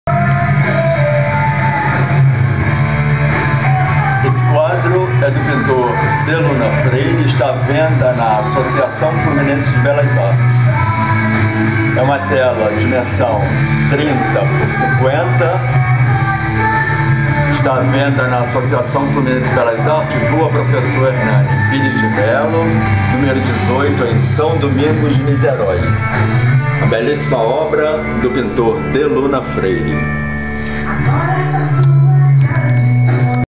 bbb comendador delunafreire
obra do artista plástico delunafreire quadro charitas a venda na AFBA.